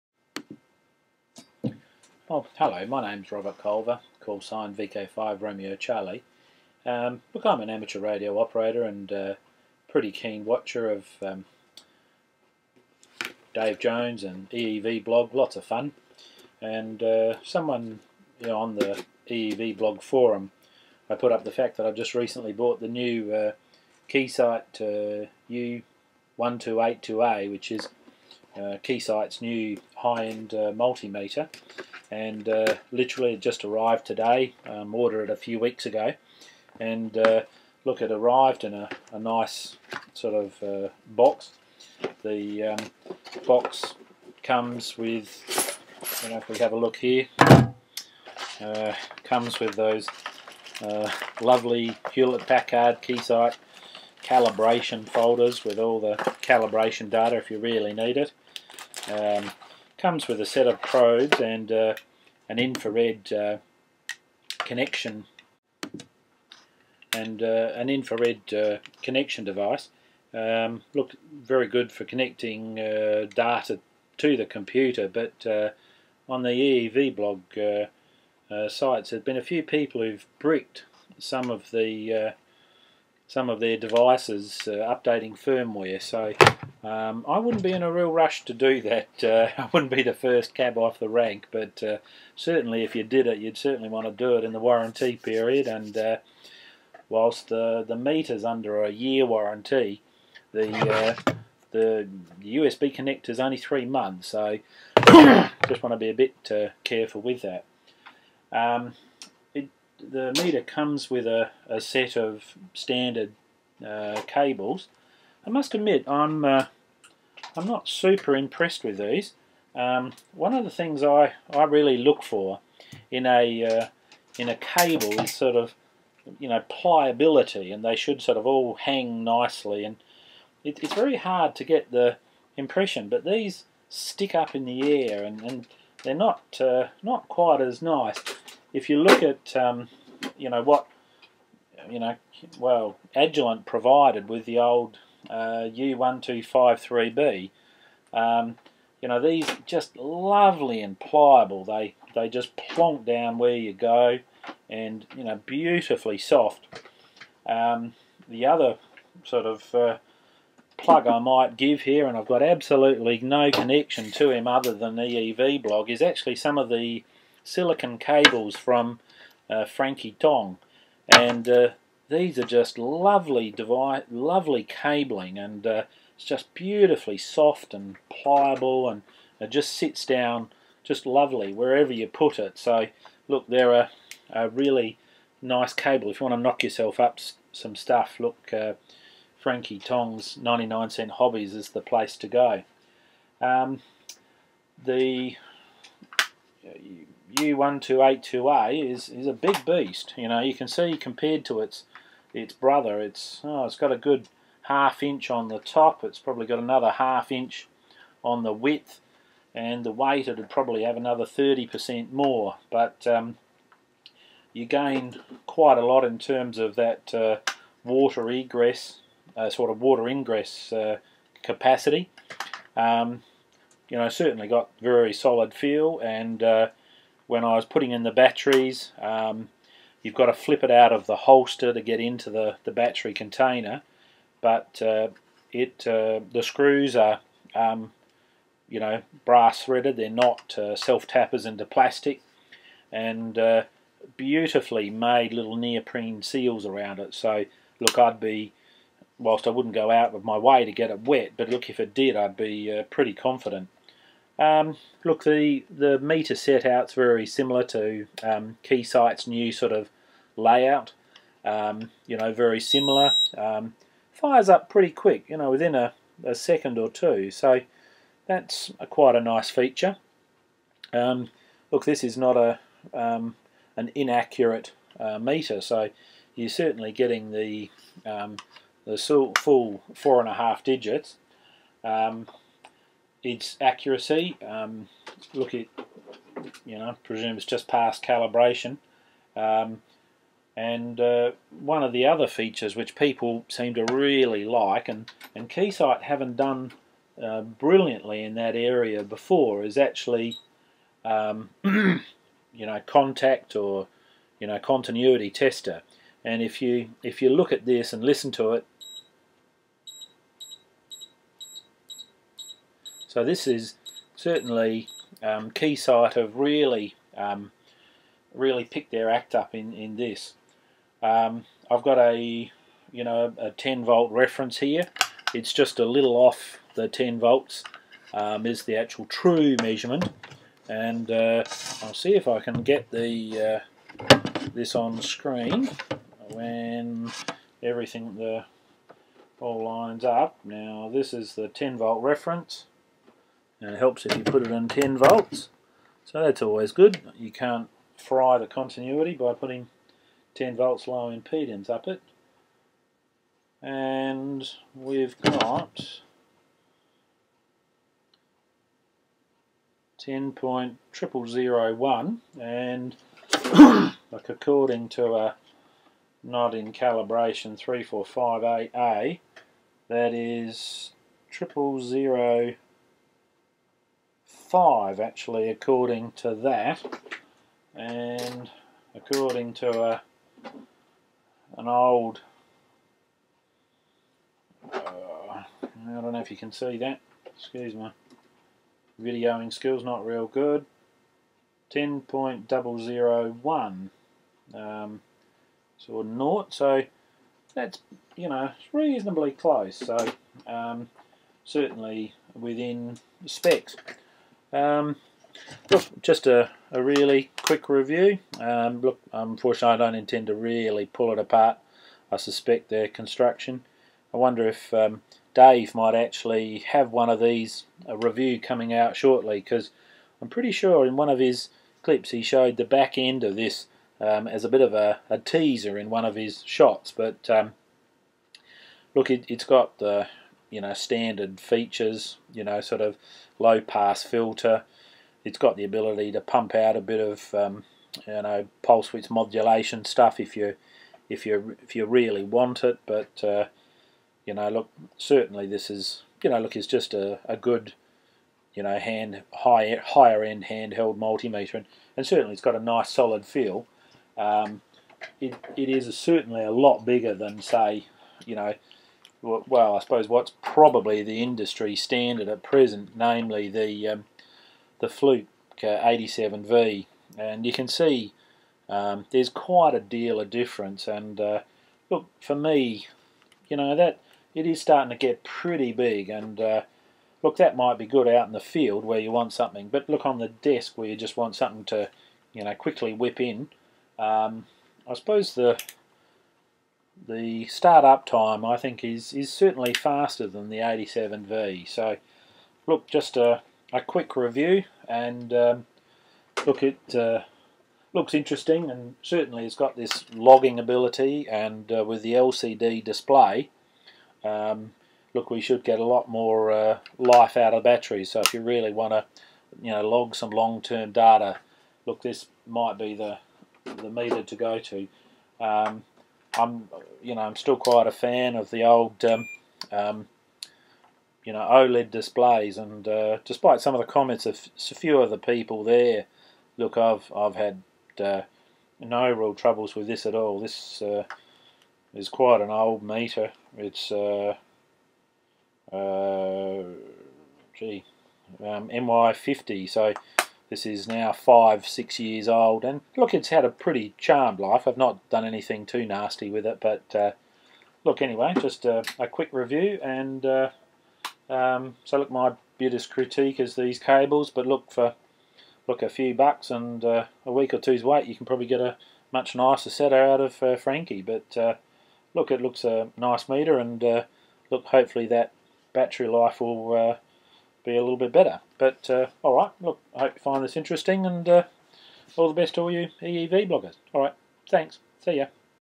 Quick Review Keysight U1282A
A brief open the box review of the new Keysight U1282A and first impressions.